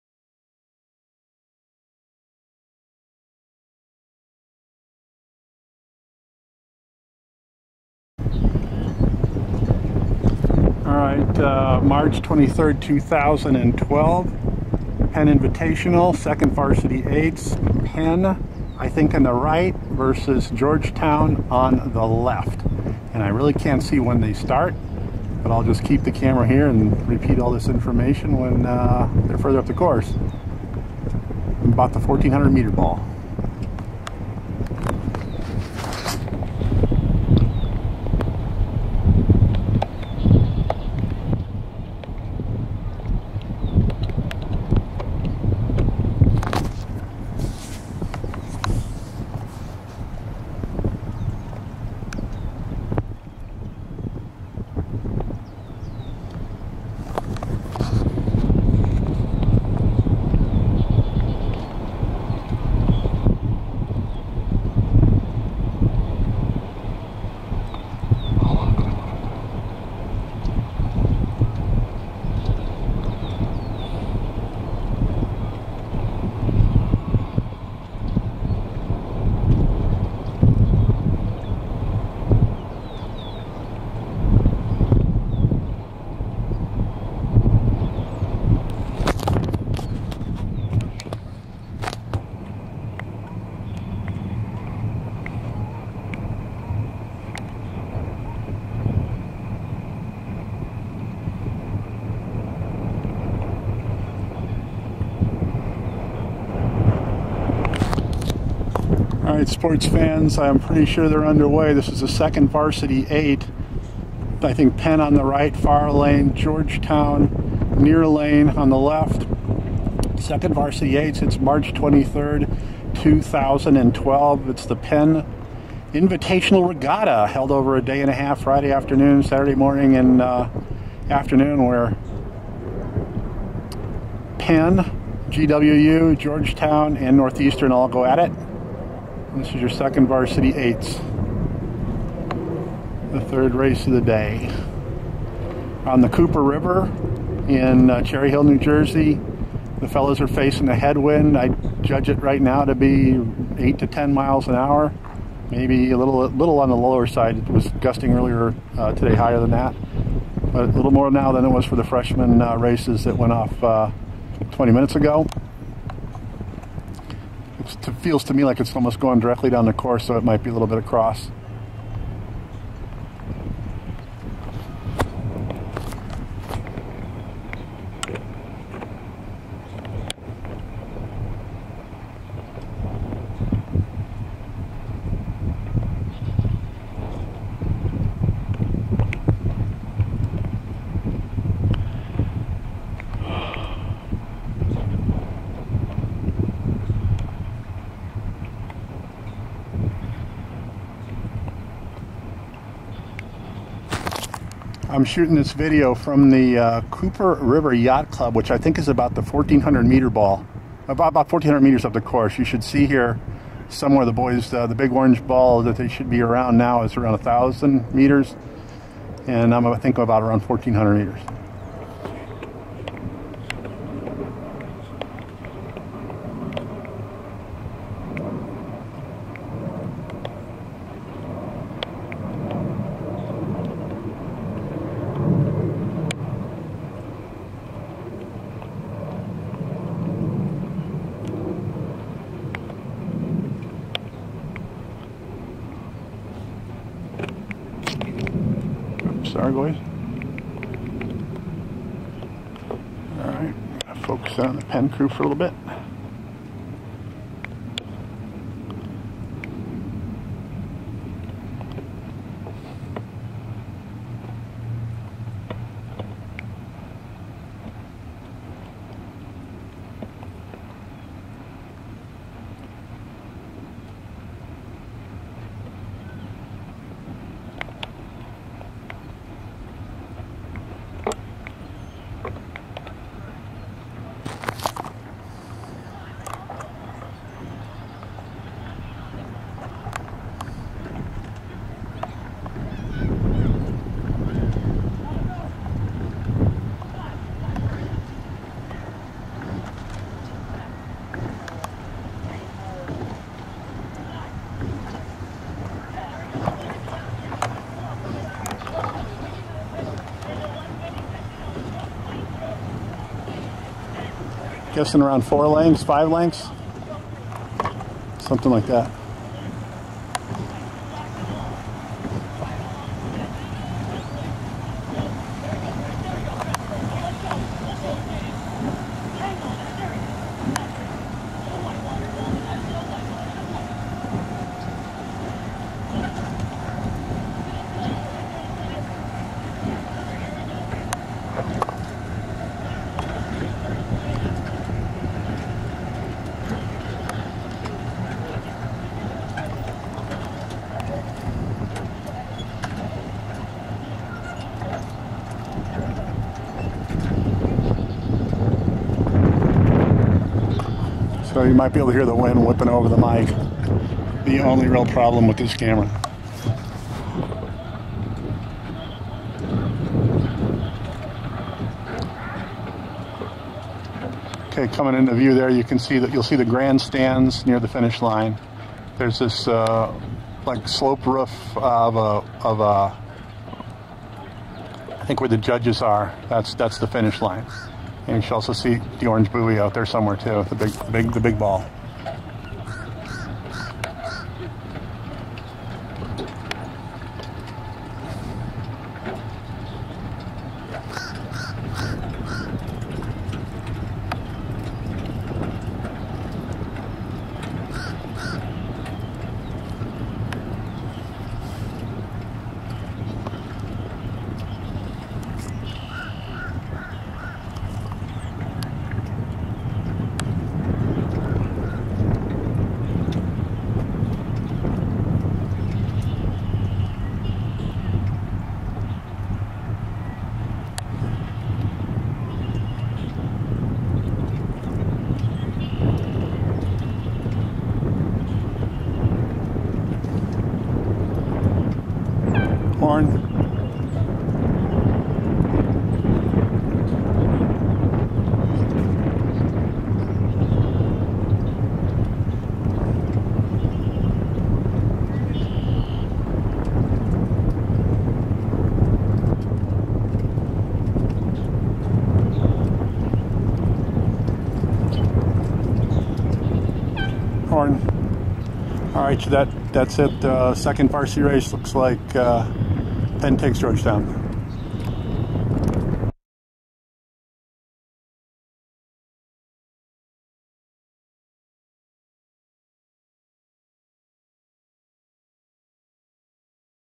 2012 EARC HM 2V8+ Penn Georgetown Crew Rowing
March 23, 2012, race time 14:20. Cooper River, Cherry Hill, NJ. Heavyweight Men, 2nd Varsity 8+. Penn 6:33.2, Georgetown 6:57.6.
Pennsylvania Invitational Regatta.
Weather: 78F. Headwinds from 5-10 mph from East to West.
Camera at ~1400m on South shore at Cooper River Yacht Club dock.